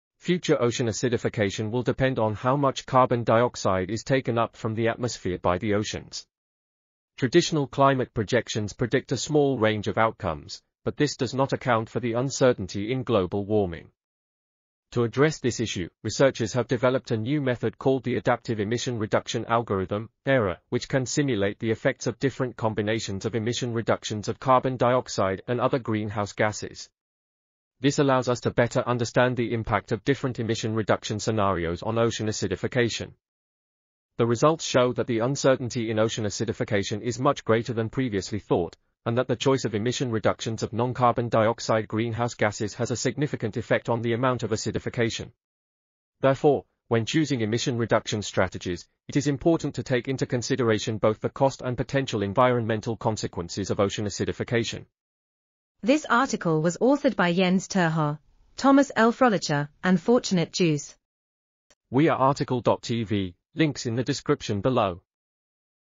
Ocean acidification in emission-driven temperature stabilization scenarios: the role ... | RTCL.TV
### Keywords ###

### Article Attribution ###
Title: Ocean acidification in emission-driven temperature stabilization scenarios: the role of TCRE and non-CO2 greenhouse gases
Authors: Jens Terhaar, Thomas L Frölicher ,and Fortunat Joos
Publisher: IOP Publishing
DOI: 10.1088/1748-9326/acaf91

### Video Timestamps ###
0:00:00 - Summary
0:00:55 - Title
0:01:01 - Outro
0:01:05 - End